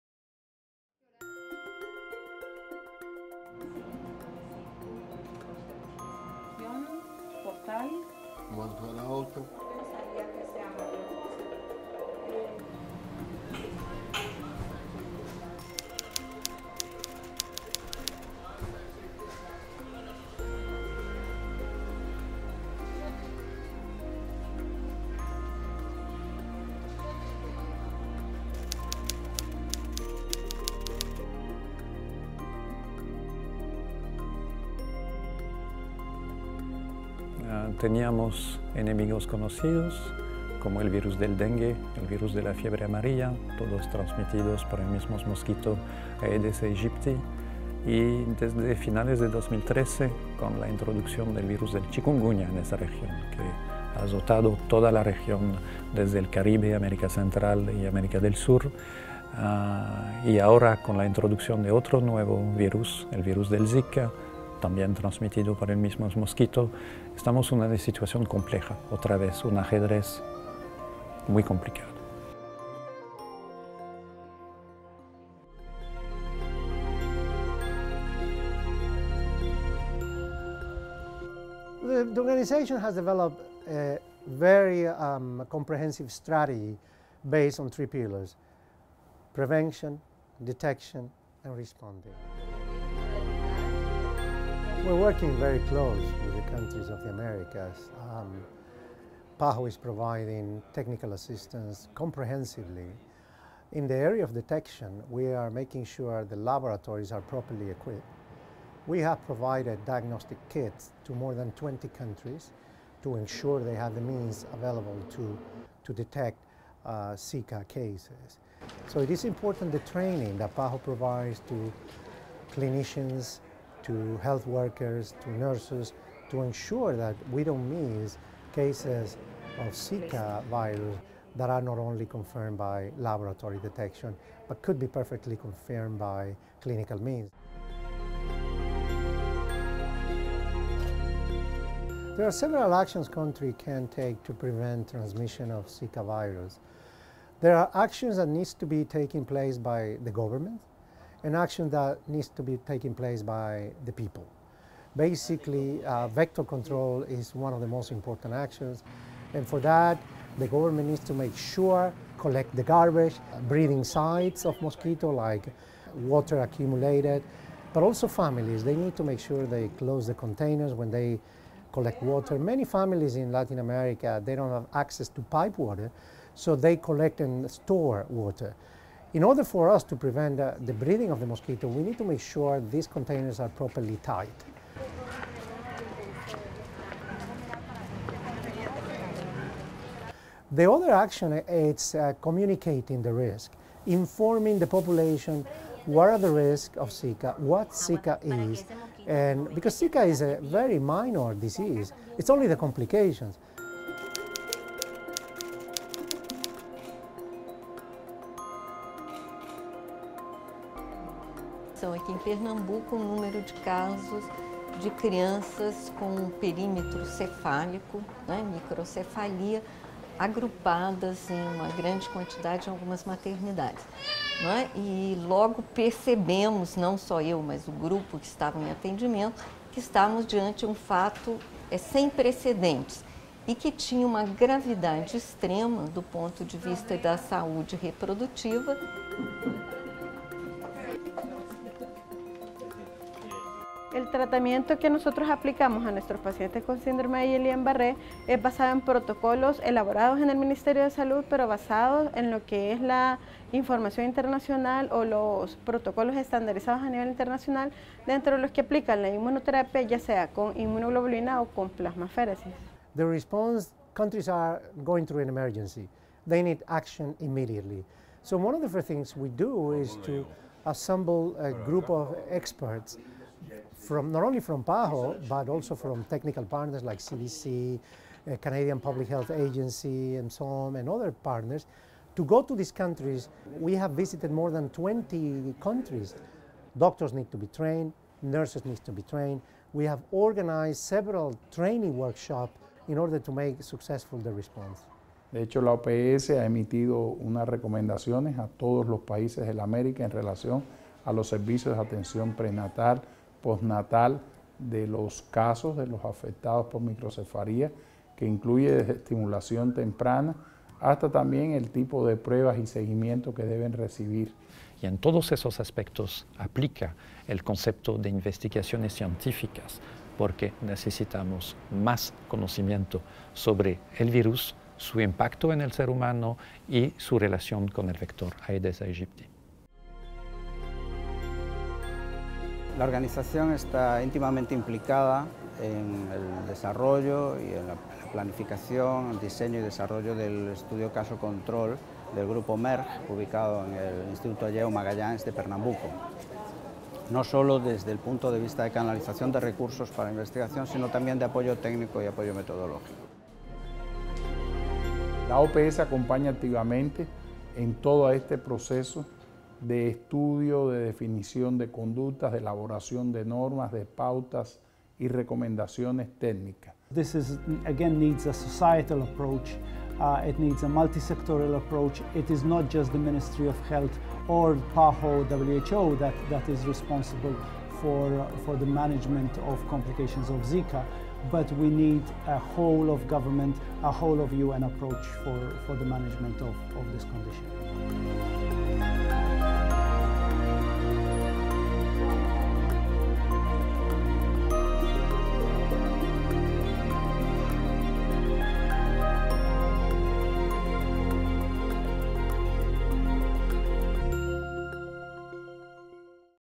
PAHO/WHO's response to the Zika outbreak
Know more about what PAHO/WHO is doing to respond to the zika outbreak in the Americas